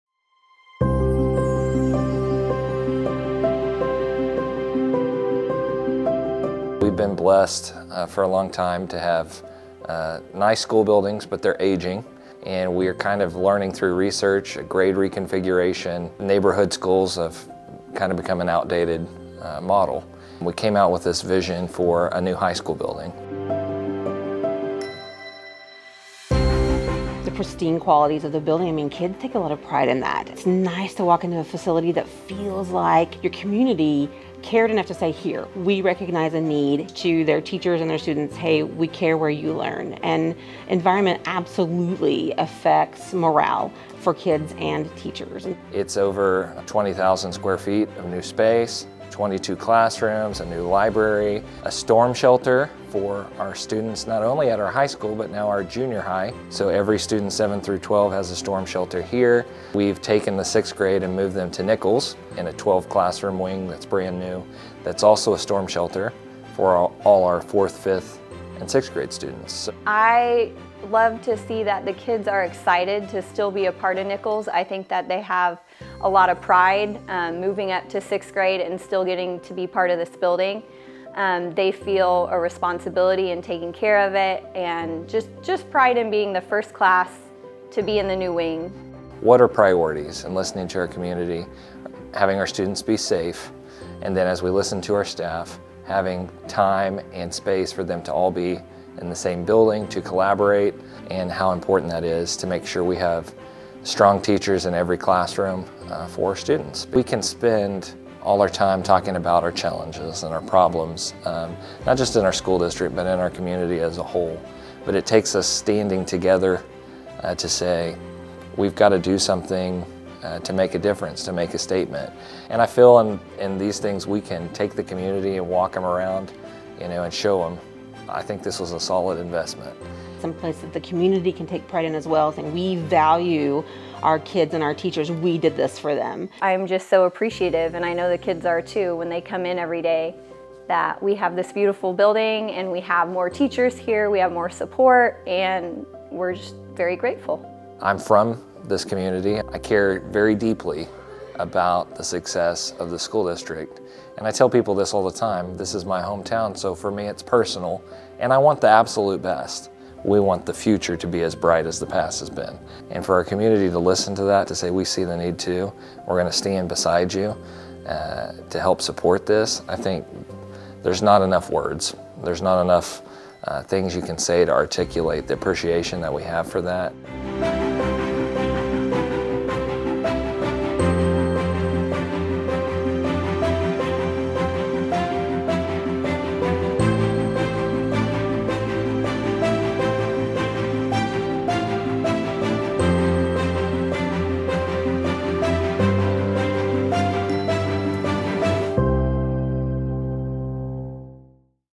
Thank You Miami | Miami Public Schools Bond Issue 2022
In 2019, our community joined together to vote for major improvements to our school district.

A brand new high school facility, a reconfigured Miami Junior High School on the old high school property, and a new 6th-grade wing at Nichols Elementary were the result of this bond issue.

We want to say "Thank You" to the citizens of Miami, OK for supporting education and believing in our leadership.